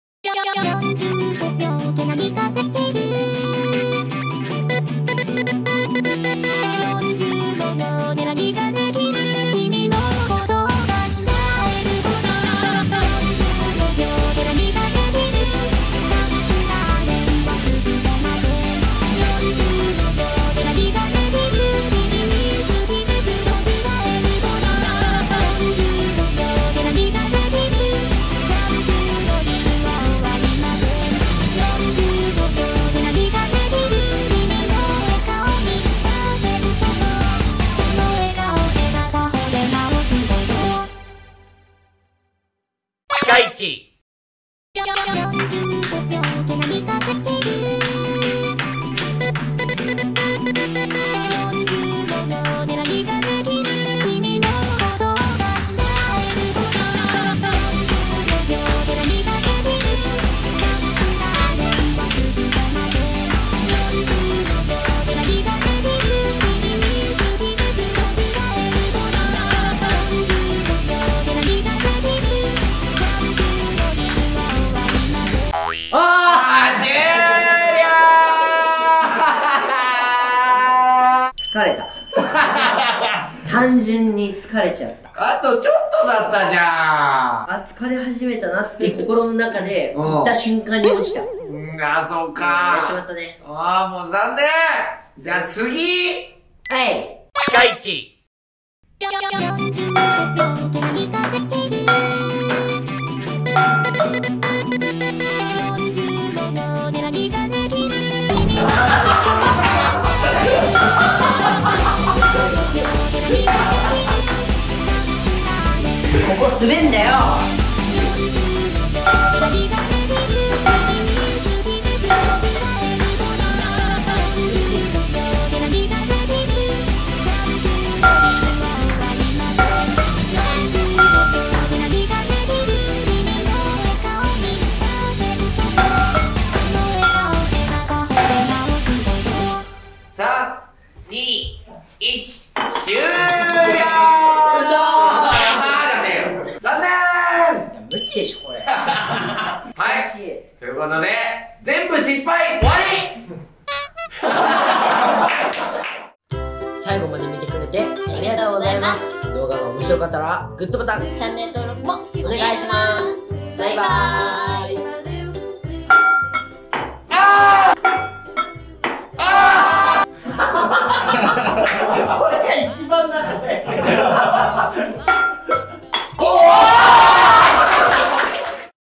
鈴木福が45秒踊ってみた〜ついでにけん玉＆ピンポンチャレンジ〜
※緊急事態宣言解除後の撮影につきまして
検温・手指の消毒の義務化や換気の徹底など、感染拡大の防止対策に取り組んだ上で撮影を行っております。

45秒で何ができる？？🤔

鈴木福が45秒でけん玉・ピンポン玉チャレンジに挑戦⚡️⚡️
ダンスも必見🕺

▽ひまちゃんver.

【ピカいちCHANNEL】
毎週月曜、水曜、金曜の18時を目安に動画配信中！

撮影の裏側もわかる公式アカウント